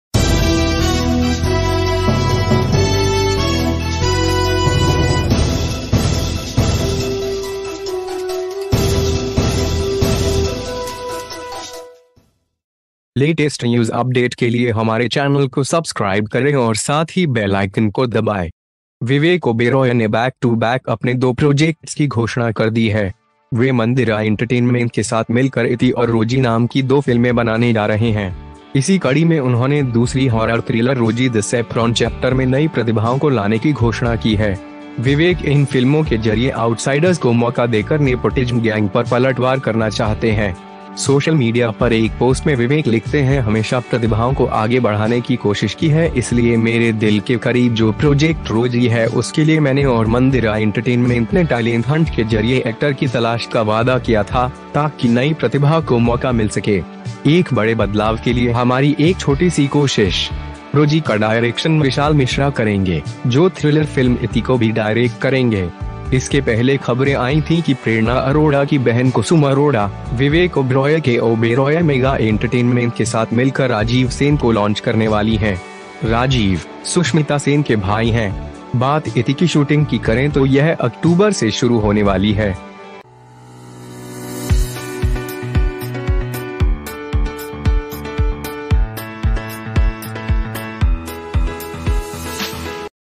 मनोरंजन विवेक ओबेरॉय फिल्म रोजी के लिए टैलेंट हंट से चुनेंगे एक्टर : पोस्ट में लिखा- बड़े बदलाव ...
Subscripbe to VR NEWS for daily news and updates. Entertainment Vivek Oberoi to choose from Talent Hunt for the film Rosie Actor wrote A small effort for big change. Vivek Oberoi has announced his two projects back to back. He is teaming up with Mandira Entertainment to produce two films, namely Iti and Rosie. In this episode, he has announced the introduction of new talent in the second horror-thriller Rosie the Saffron Chapter. Vivek wants to counter the nepotism gang by giving opportunities to outsiders through these films. Vivek writes in a post on social media- has always tried to pursue talent, so for the project Rosie which is close to my heart, I and Mandira Entertainment promised to find an actor through talent hunt, so that new talent To get a chance. Our little effort for a big change. Rosie will be directed by Vishal Mishra, who will also direct the thriller film Iti. Earlier there were reports that Prernaa Arora sister Kusum Arora, along with Vivek Oberoi Oberoi Mega Entertainment, is about to launch Rajiv Sen. Rajiv is the brother of Sushmita Sen. Talk about shooting of Iti, it is going to start from October.

VR NEWS !!